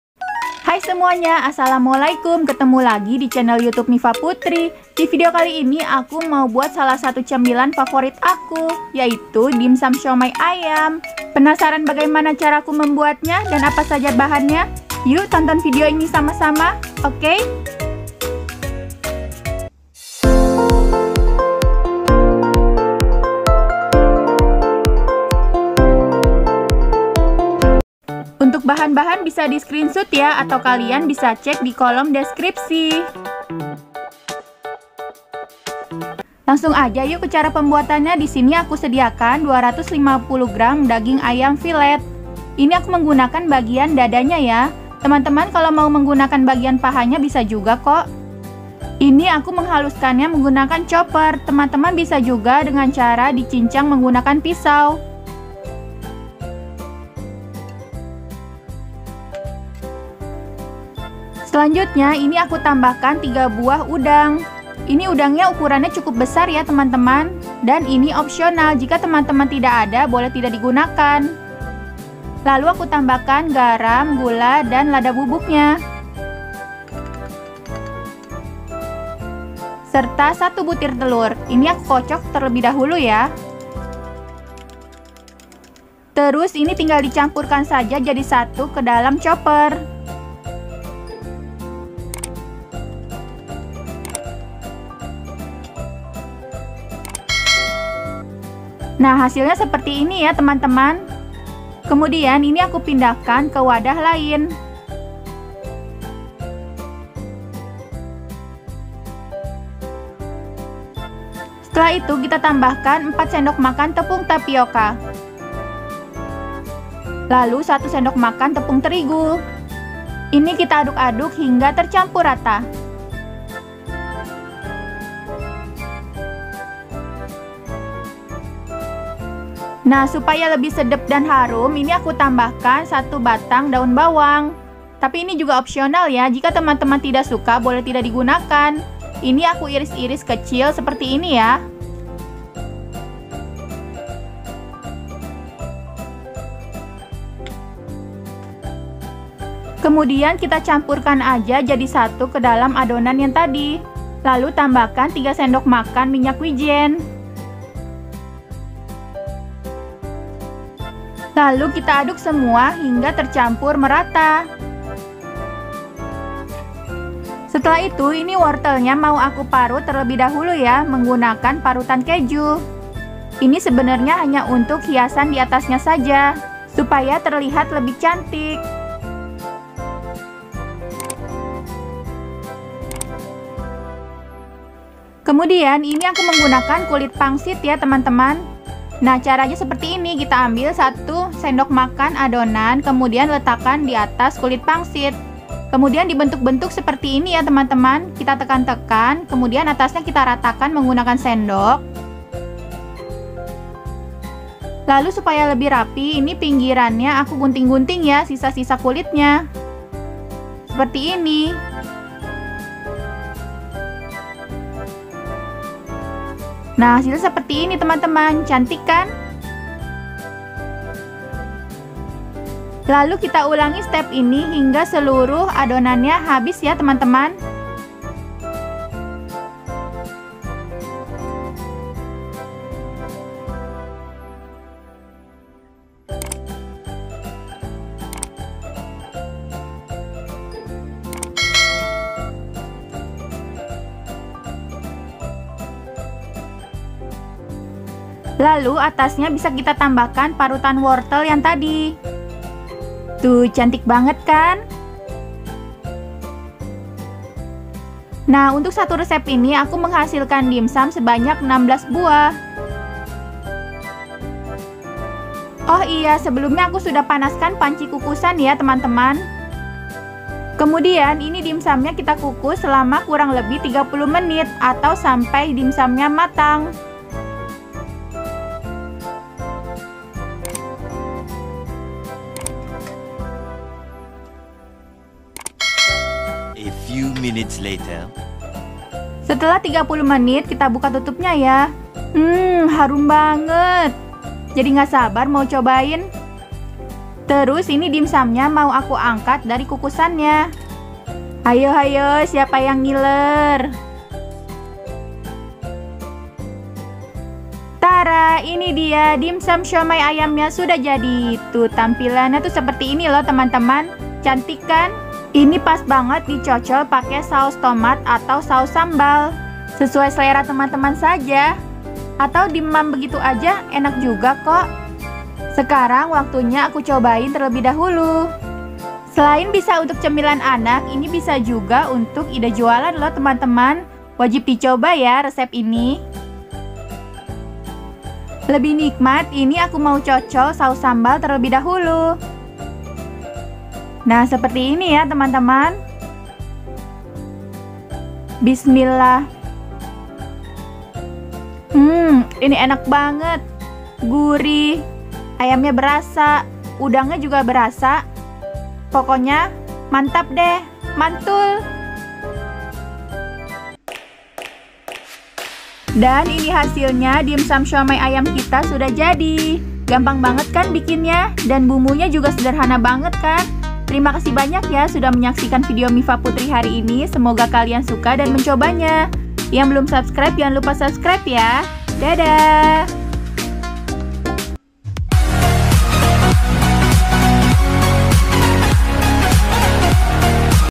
MURAH DAN SIMPLE! suami ku selalu meminta DADA AYAM di Masak seperti ini! DIMSUM AYAM
Dipublikasikan tanggal 09 September 2021

Bahan-bahan sebagai berikut :
250 gr Daging Ayam Fillet
3 buah Daging Udang Besar
4 sdm Tepung Tapioka
1 sdm Tepung Terigu
1 sdt Garam
1/2 sdt Merica Bubuk
1 sdt Gula Pasir
1 buah Wortel
1 butir Telur
3 sdm Minyak Wijen
1 batang Daun Bawang

Nah untuk cara membuatnya tonton terus videonya yaa..

Terima kasih.